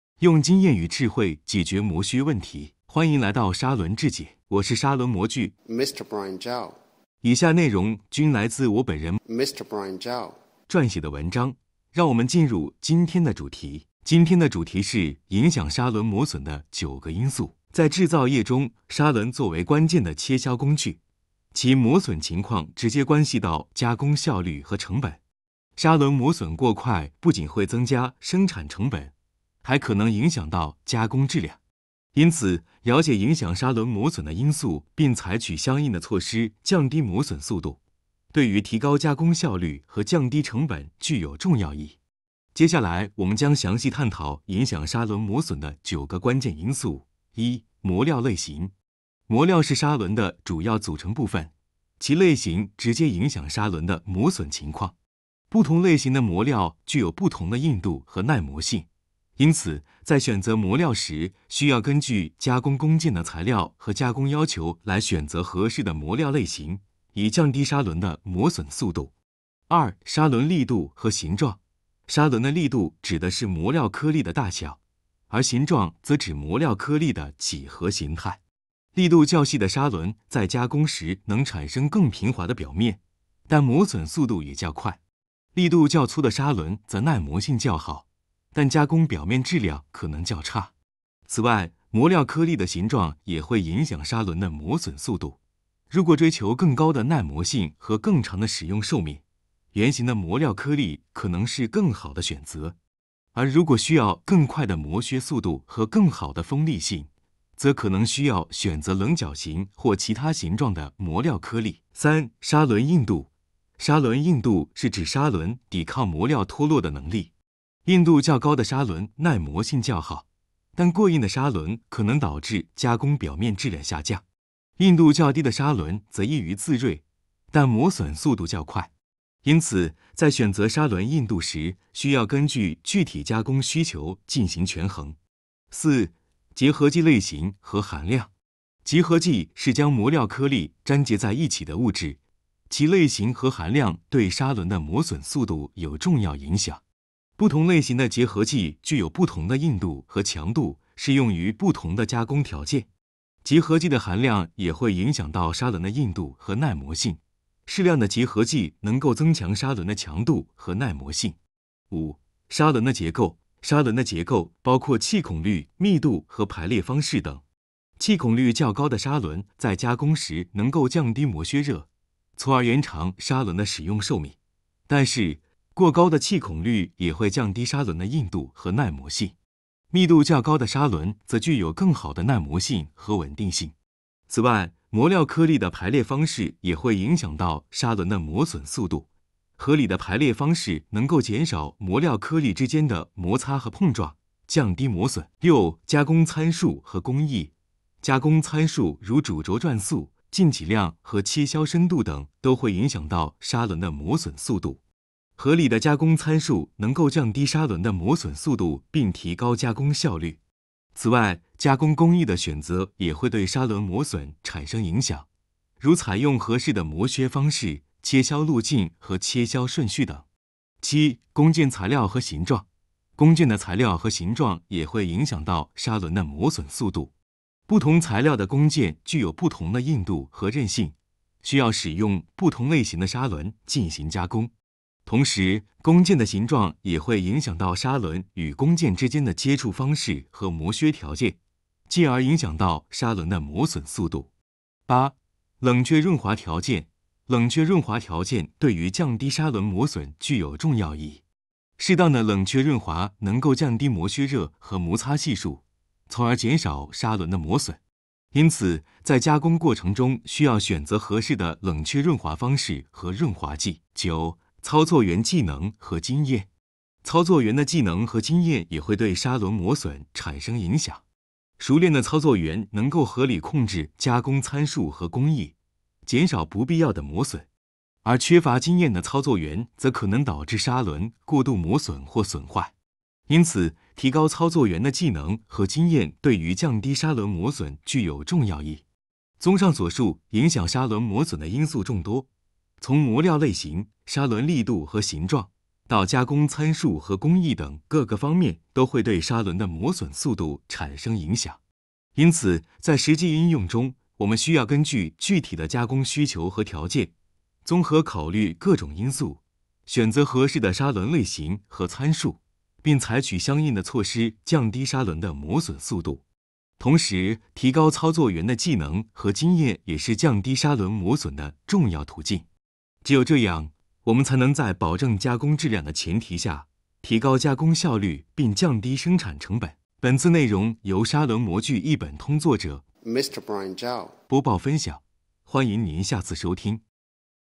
影响砂轮磨损的九个因素
砂轮作为制造业关键切削工具，其磨损影响加工效率和成本。影响砂轮磨损的九个关键因素，包括磨料类型、砂轮粒度和形状、硬度、结合剂、结构、加工参数、工件材料和形状、冷却润滑条件以及操作员技能。合理选择砂轮和参数，提高操作员技能，有助于降低磨损。砂轮